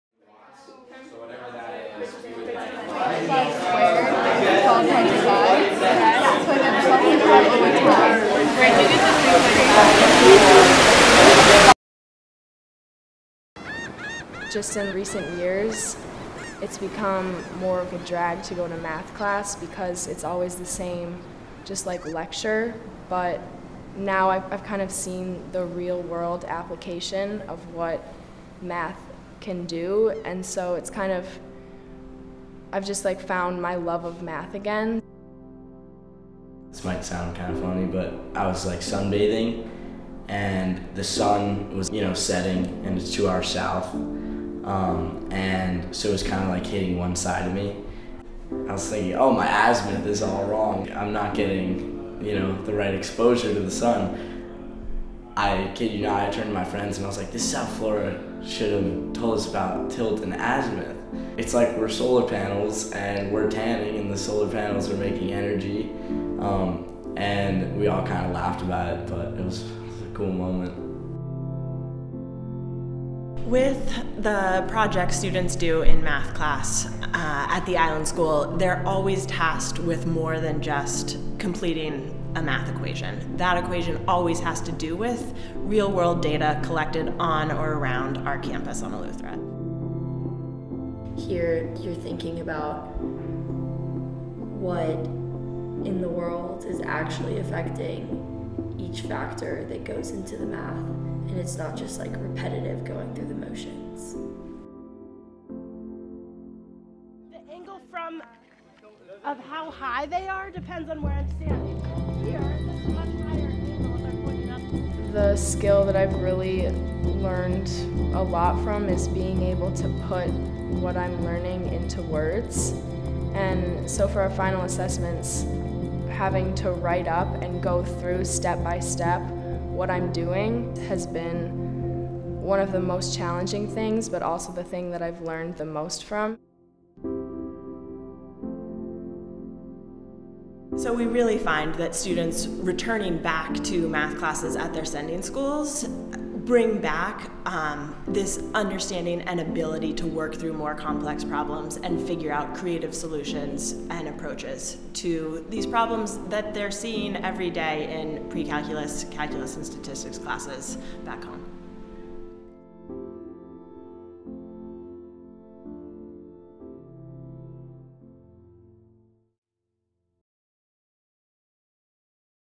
Applied Mathematics
Is math a daunting subject for you? Take a look at how we approach it at the Island School.